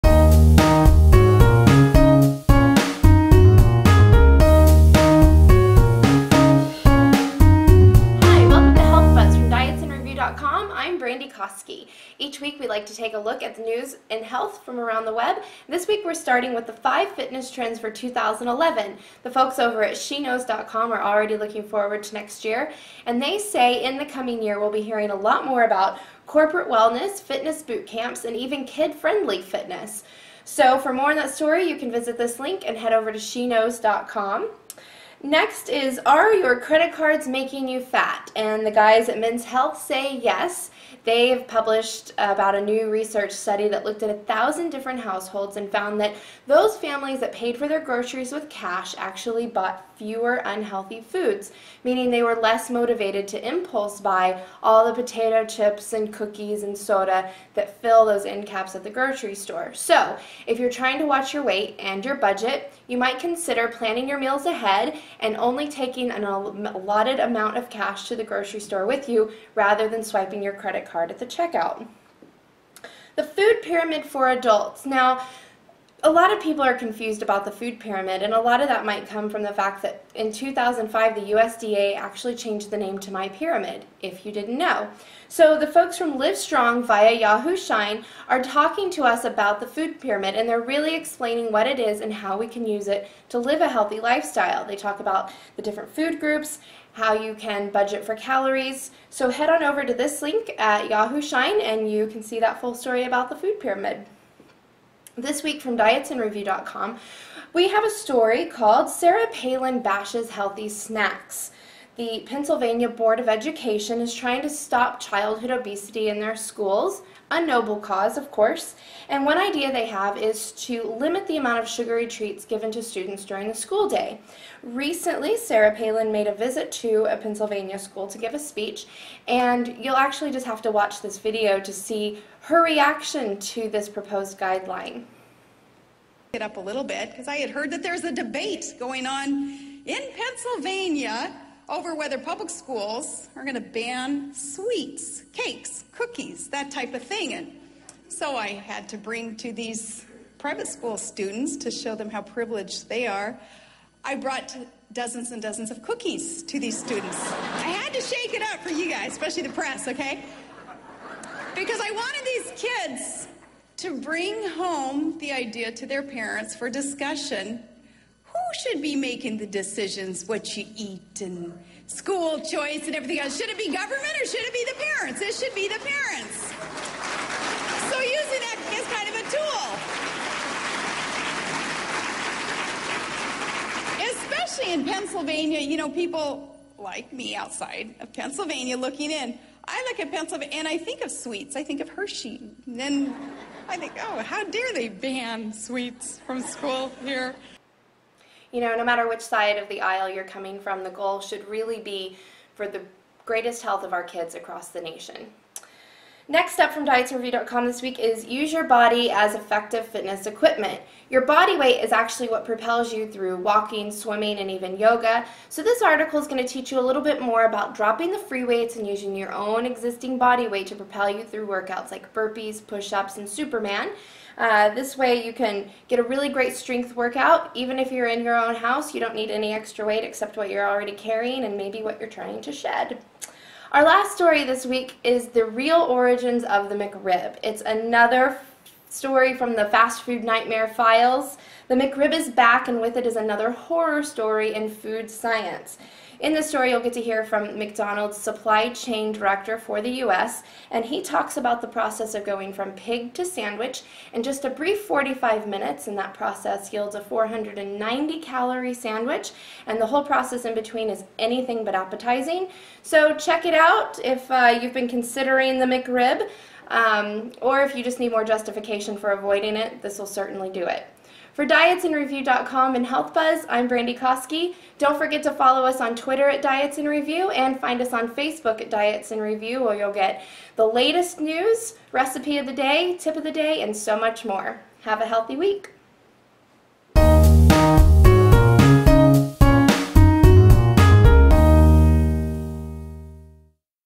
Health Buzz Episode 9
In this week's health news from around the Web, we take a look at Sarah Palin's cookie gate, how the McRib is made, and fitness trends for 2011.